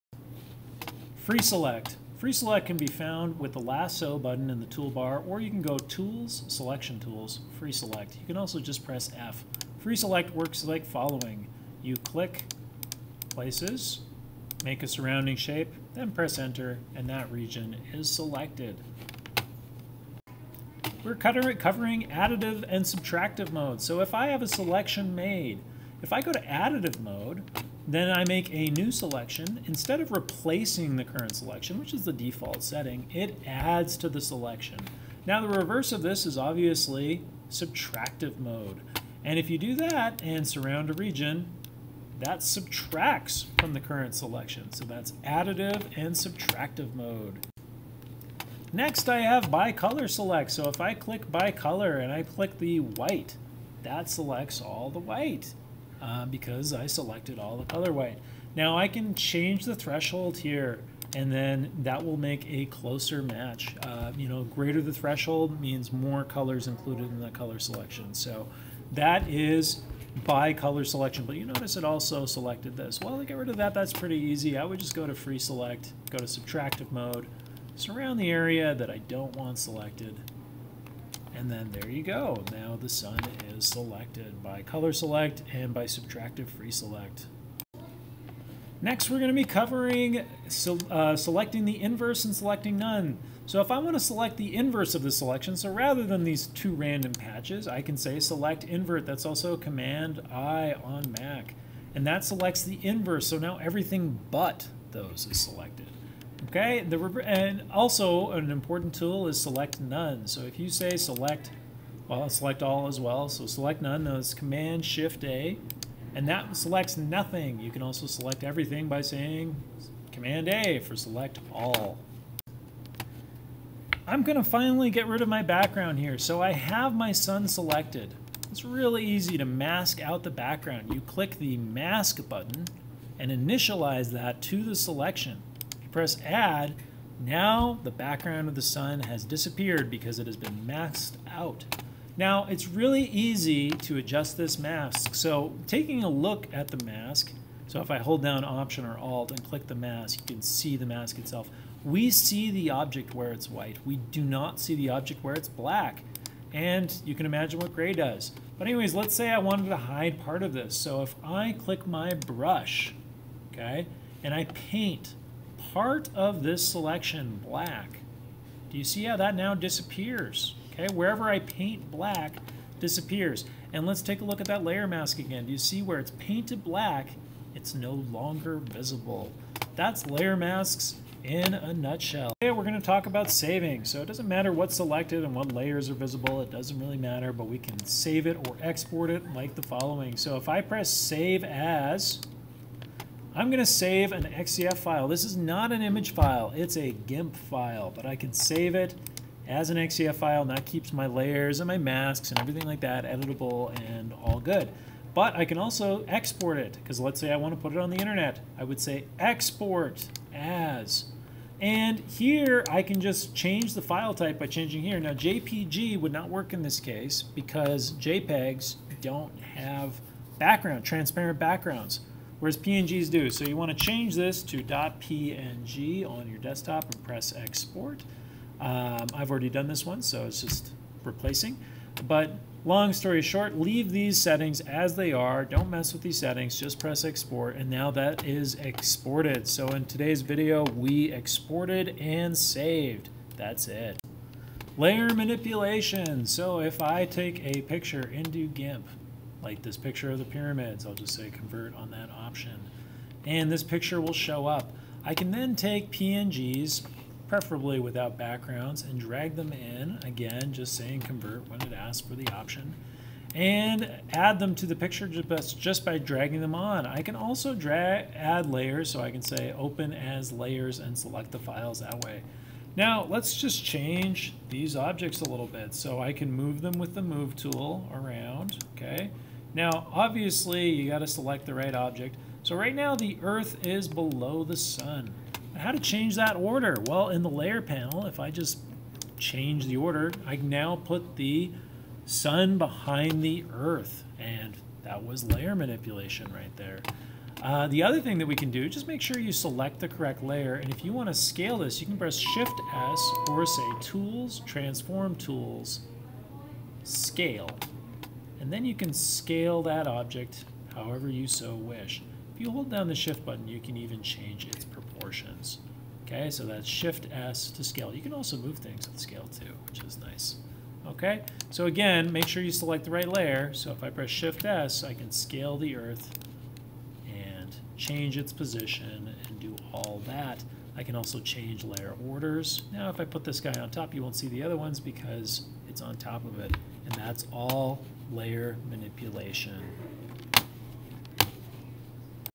GIMP Fundamentals
-Free Select 0:00
-Additive/Subtractive Selection Modes 0:20
-By Color Select 0:48
-Select Inverse/None 1:31
-Creating a Layer Mask  2:02
-Saving & Exporting 3:02
-Layer Manipulation 4:04